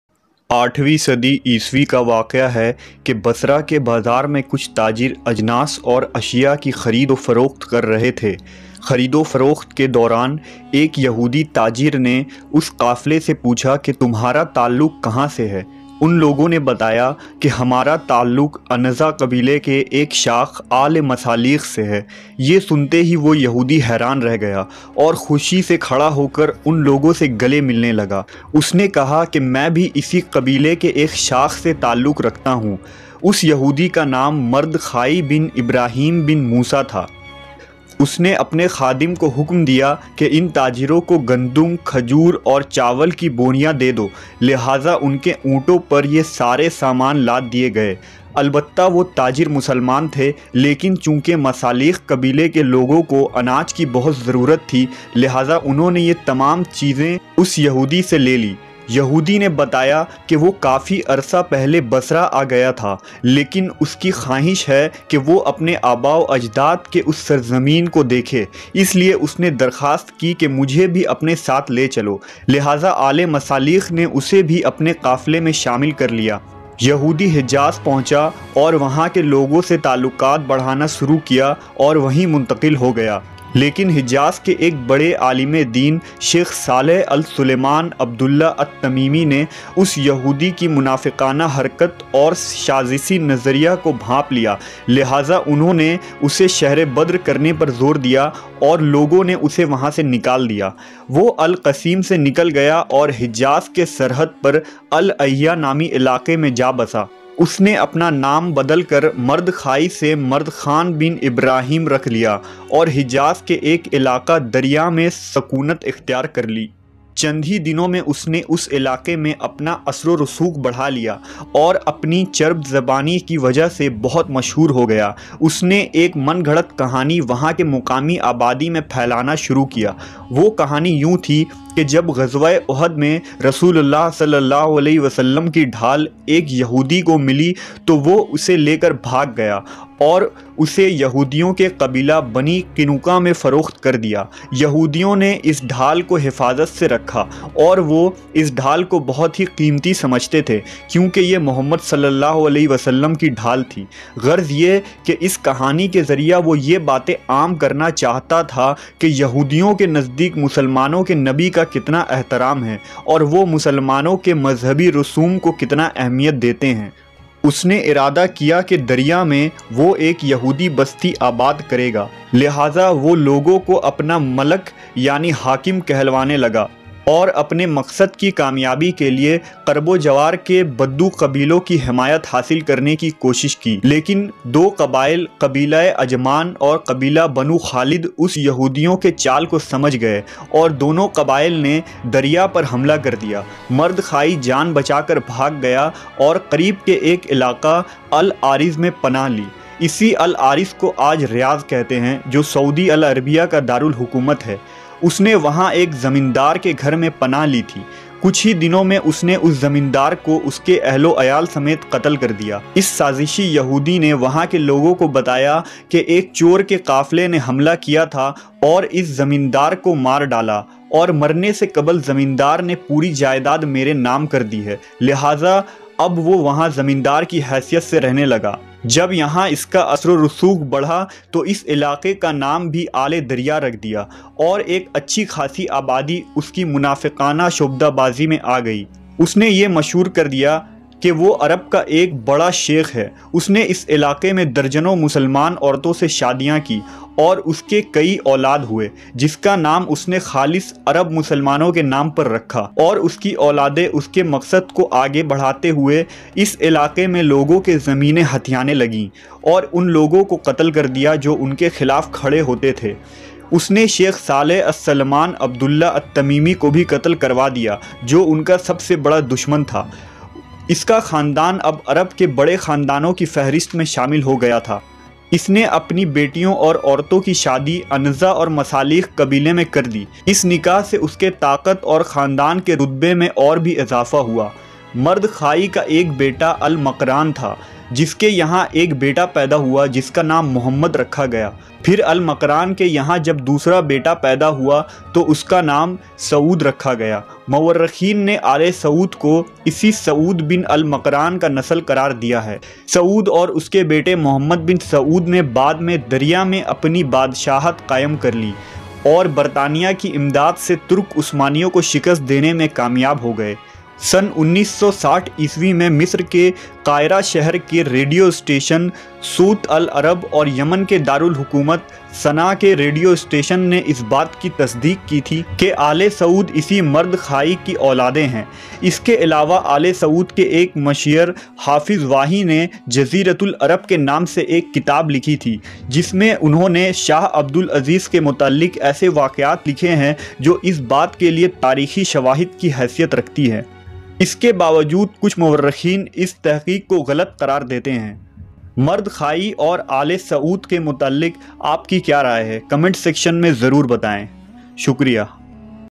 History and Reality of Aal e Saud Family | Saudi Arab | Najd | Wahabiyat | Amber Voice | Urdu Hindi
The House of Saud(Aal-e-Saud) is the ruling royal family of Saudi Arabia.
They are descendants of Muhammad bin Saud, and ibn Saud was the founder of modern Saudi Arabia.
There political system is an absolute monarch. The family in total is estimated to comprise some 15,000 members; The House of Saud has had three phases: the Emirate of Dariyah, the First Saudi State which established in 1744; the Emirate of Najd, the Second Saudi State established in 1824; and the Third Saudi State established in 1902 (present Saudi Arabia)
 The family has had conflicts with the Ottoman Empire, the Sharif of Mecca, There is a historic myth that Muhammad bin Saud was the grandson of a Jew his name was Mardkhai(Later known as mardkhan), He was a burglar and killed so many Emir of Bedouin Arab tribes.